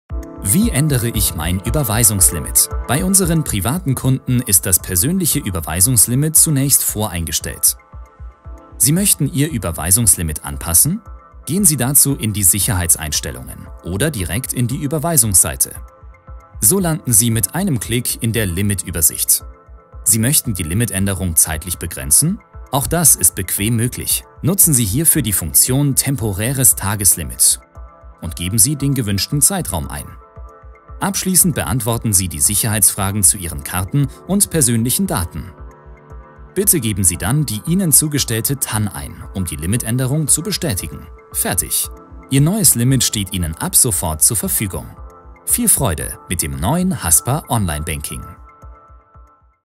Wie ändere ich mein Überweisungslimit? | Hamburger Sparkasse
Sie möchten Ihr Überweisungslimit im OnlineBanking bei der Haspa einsehen oder anpassen? In diesem Video zeigen wir, wie es geht.

Alles rund um die digitalen Services der Haspa finden Sie